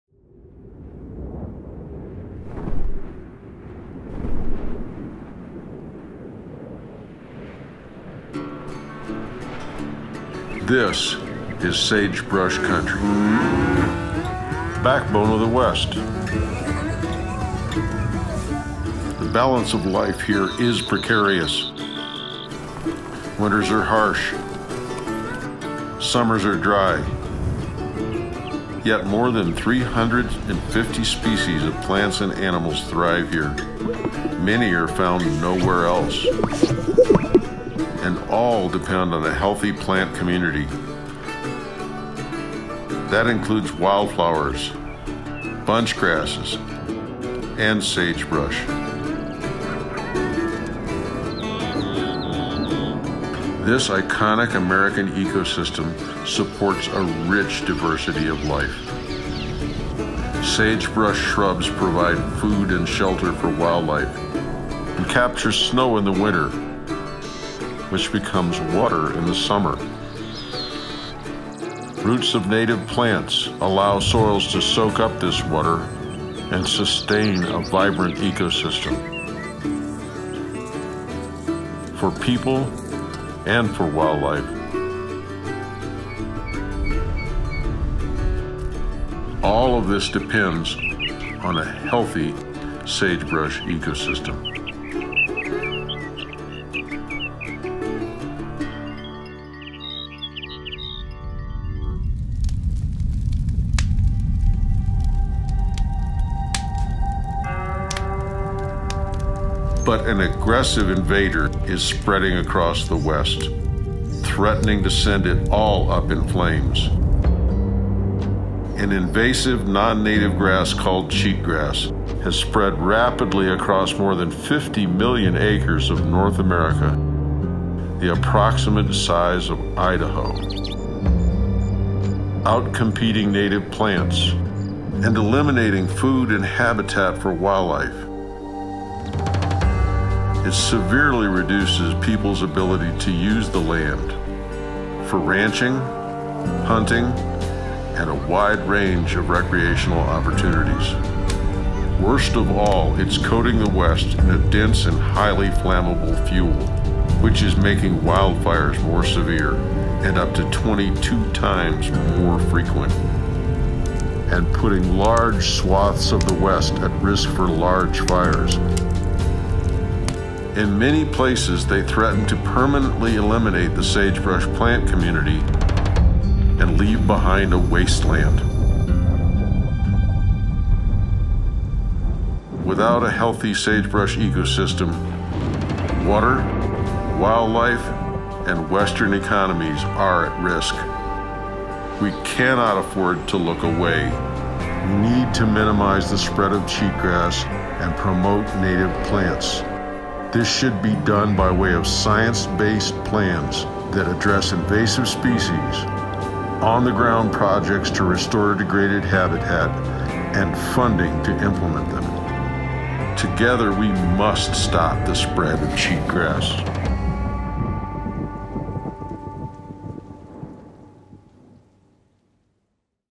Cheatgrass in Sagebrush Country: Fueling Severe Wildfires
North America’s sagebrush steppe ecosystem is home to more than 350 species of plants and animals, many of which live nowhere else. It sustains the water supply, economies, and culture of Western communities. But a deadly invader threatens to send it all up in flames. Learn how cheatgrass and other invasive weeds threaten this ecosystem’s very existence and what we must do to save it